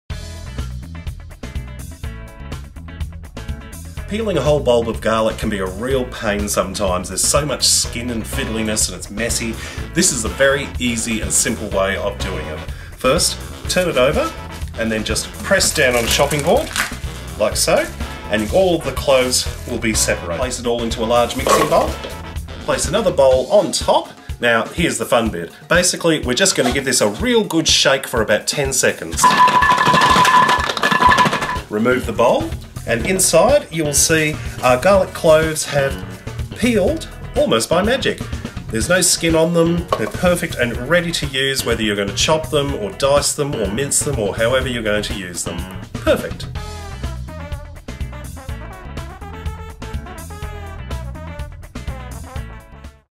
Quick Tips: Peeling Garlic in Seconds | One Pot Chef
Peel an entire head of garlic in just seconds using this simple method. It's almost magical! Never waste time on individually peeling each clove of garlic ever again :)